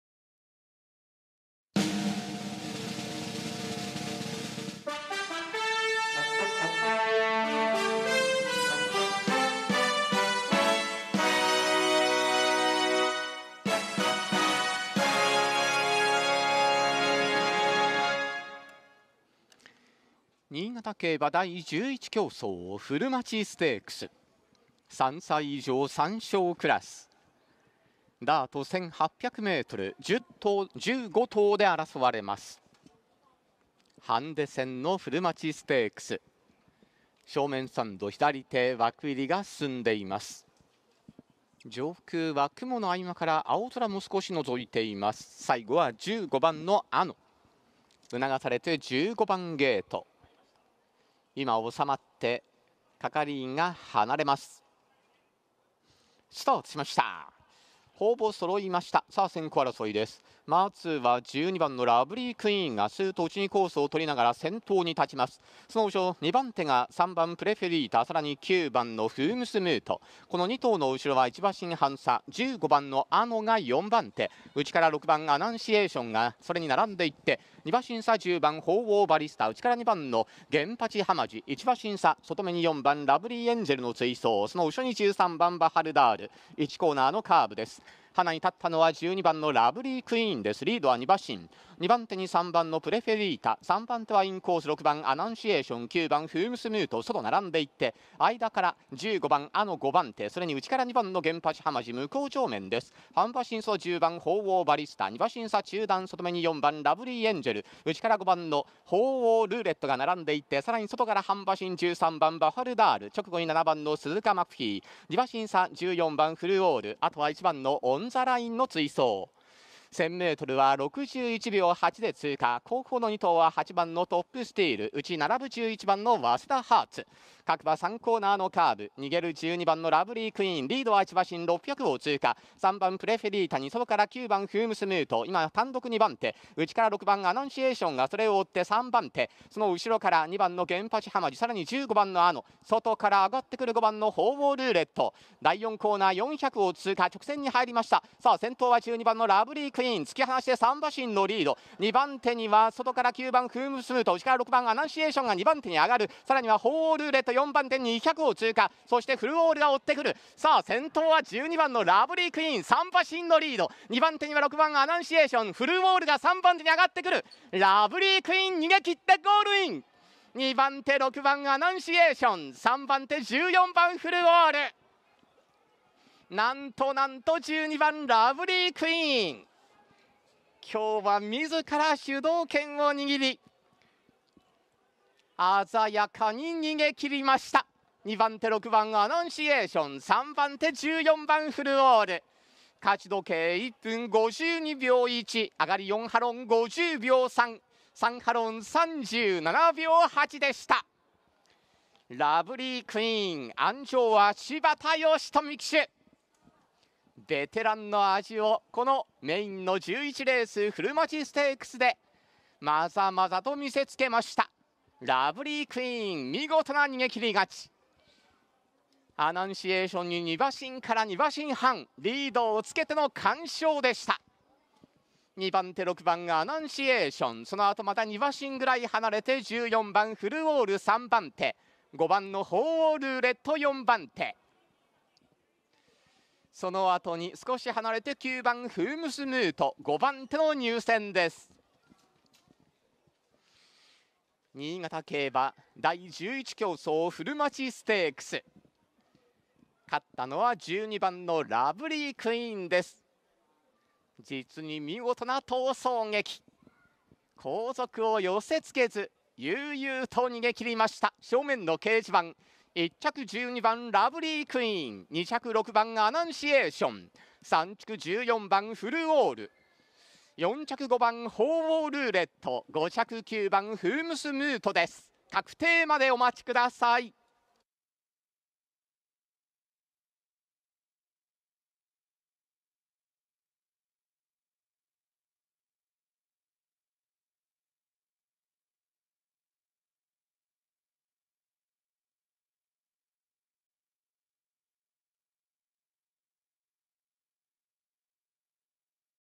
古町ステークス［2023.9.2 新潟第11R/1800m ダート］
2023年9月2日（土曜） 3回新潟7日 発走時刻：15時45分

11レース
古町ステークス

3歳以上 3勝クラス （混合）［指定］ ハンデ
コース：1,800メートル（ダート・左）